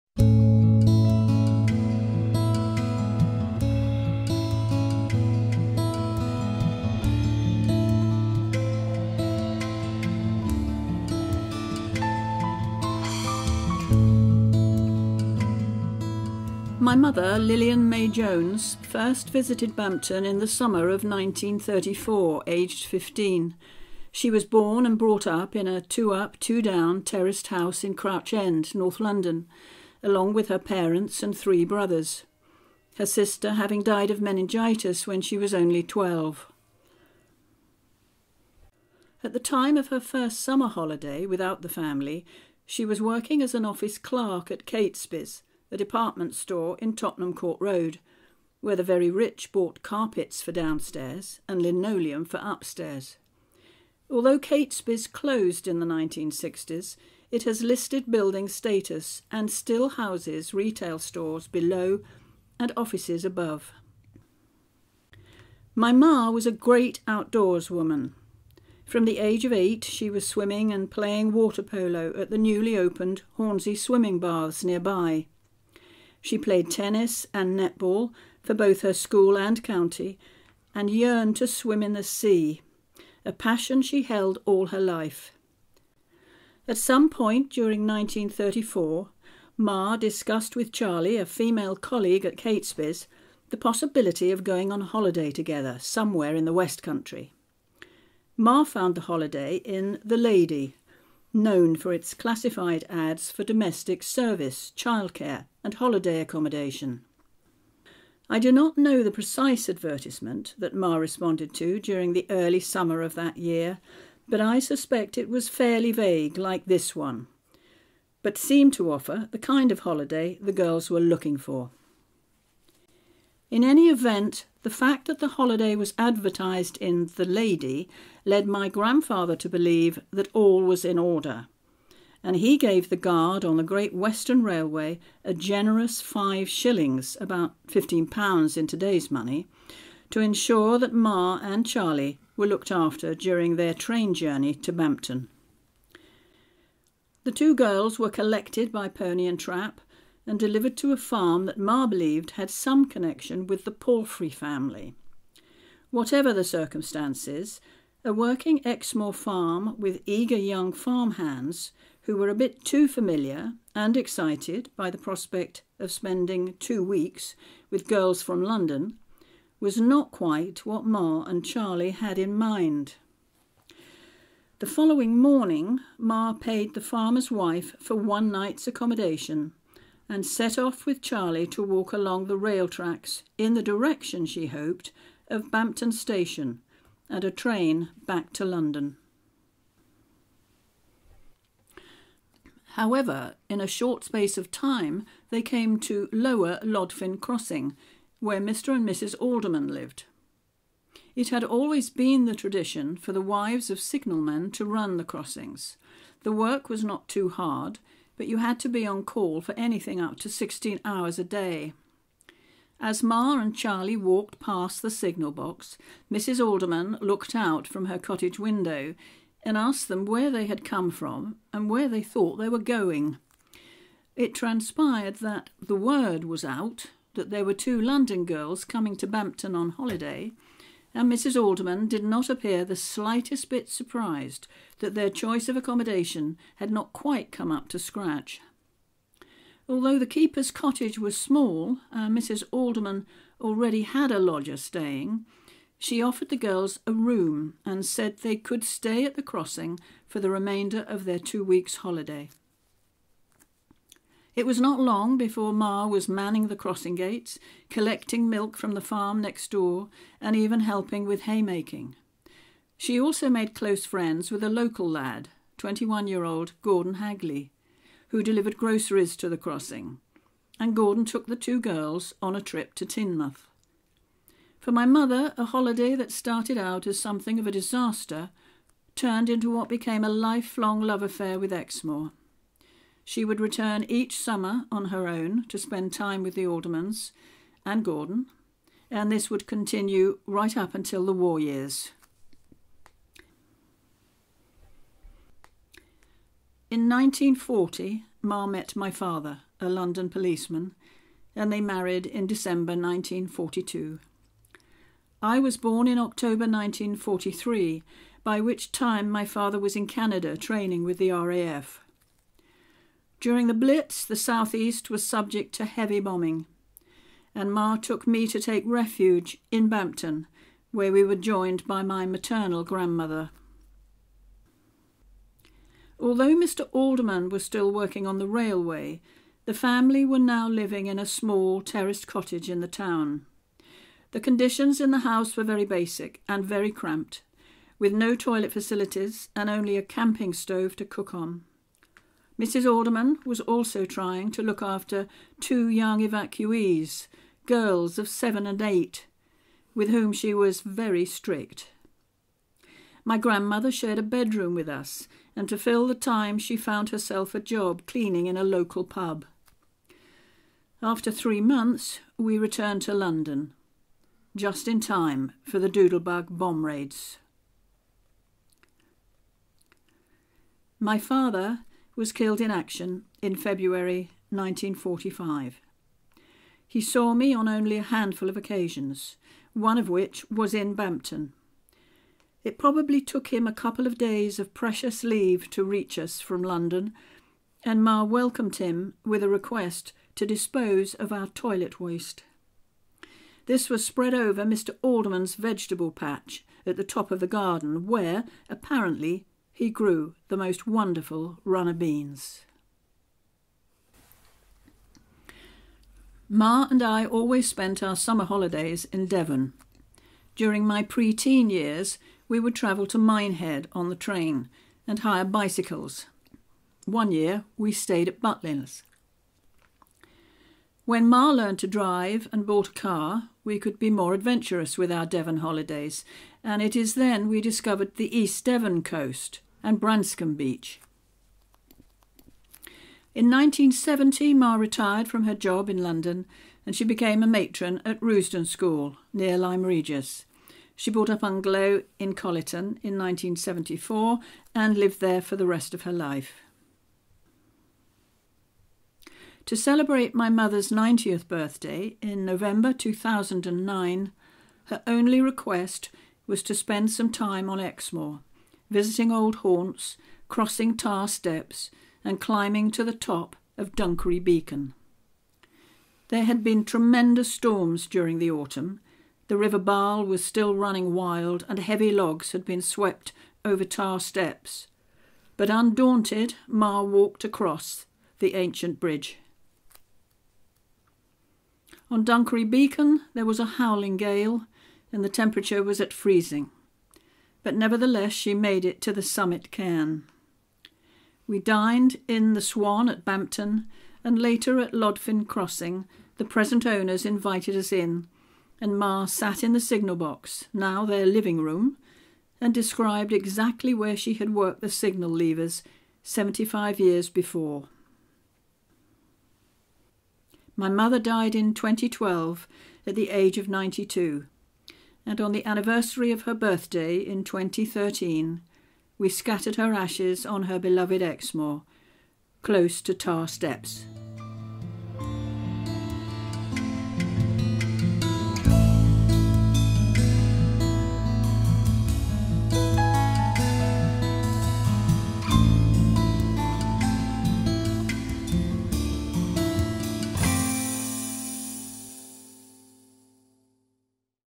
Memories of Bampton - Lilian May Jones
A very personal account of one woman's love of Bampton in Devon and of Exmoor spanning over 75 years, told by her daughter.
This video is one of a number made for the archives of Bampton Heritage and Visitor Centre.